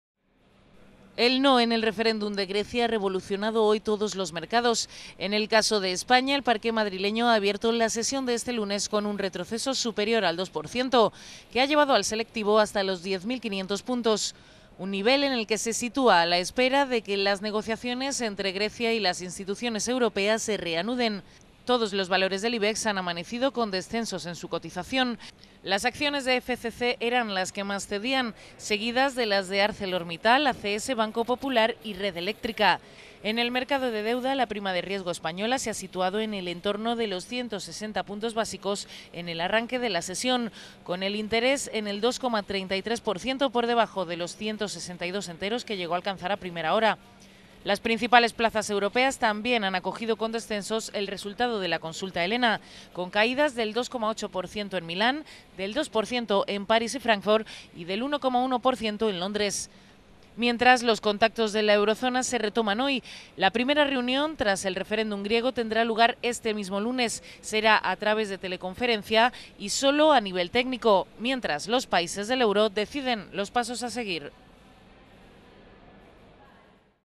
Ibex 35 se desploma un 2% tras el 'OXI' de Grecia
El 'no' en el referéndum de Grecia ha revolucionado hoy todos los mercados.En el caso de España, el parqué madrileño ha abierto la sesión de este lunes con un retroceso superior al 2%, que ha llevado al selectivo hasta los 10.562 puntos.